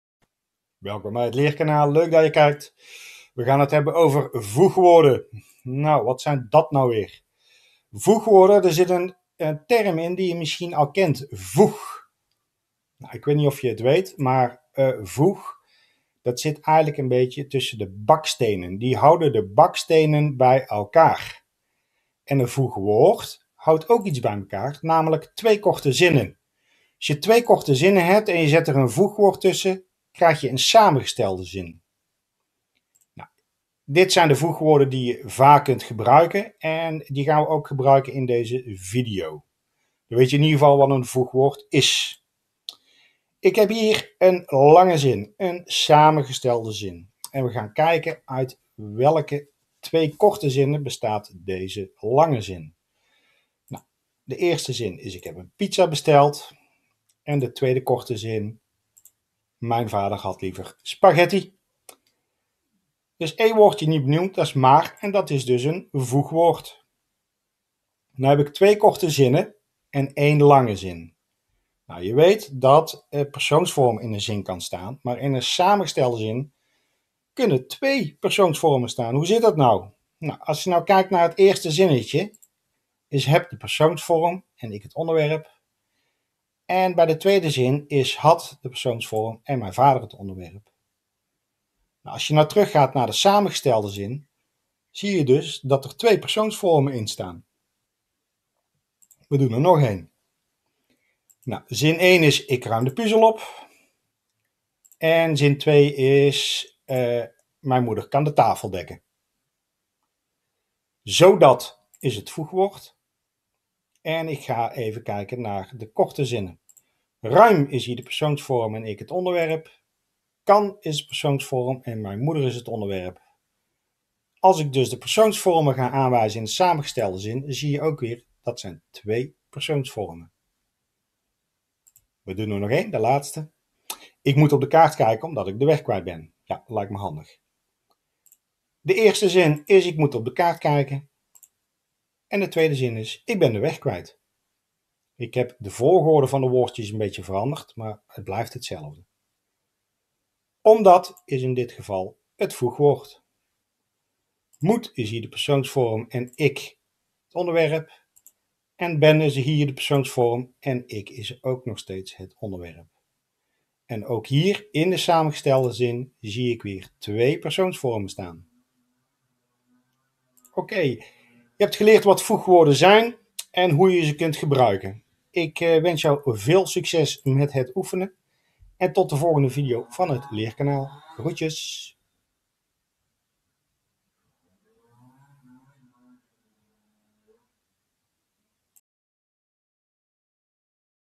Voegwoorden - Nederlandse taal
In deze video wordt uitgelegd wat voegwoorden zijn en hoe voegwoorden gebruikt worden in de Nederlandse taal.